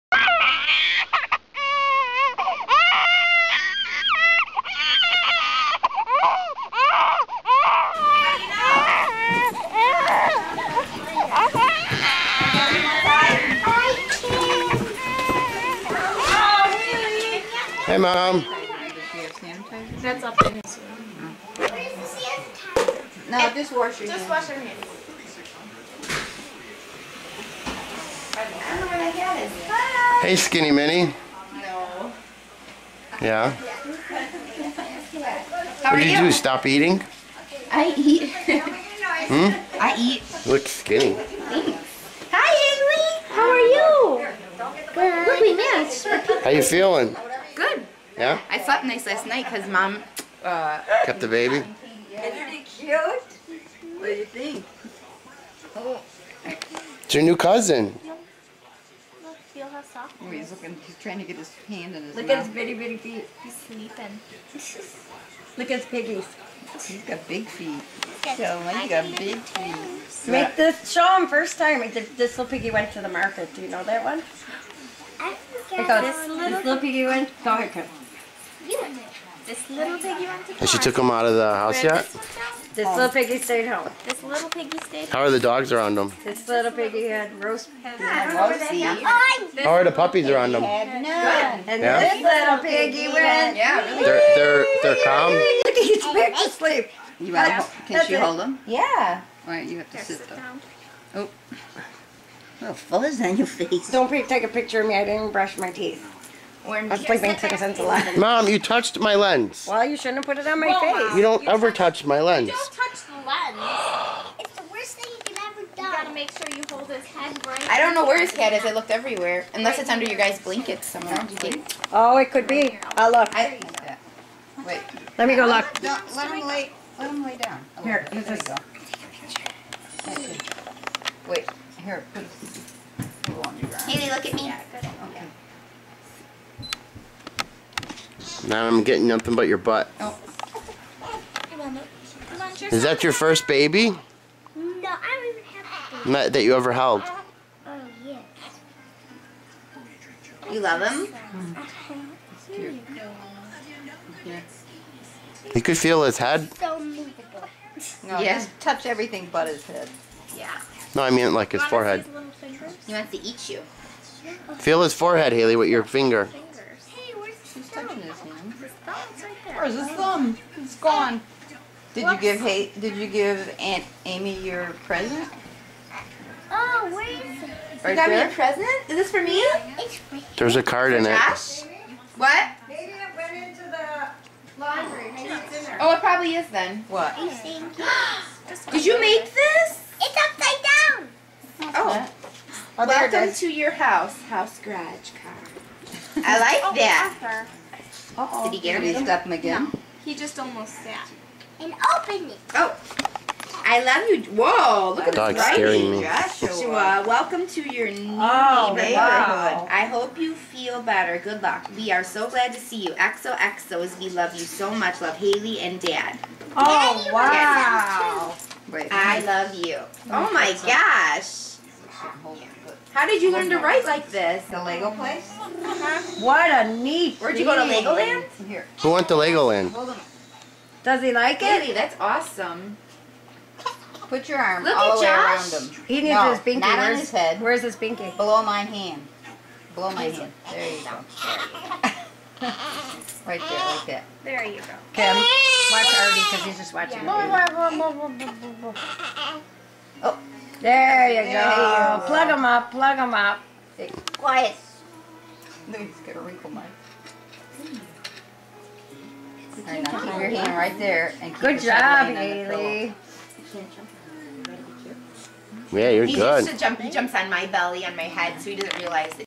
Hailey Meets Her Cousin Joshua For First time
This was March 18,2012 at my sisters house Joshuas new home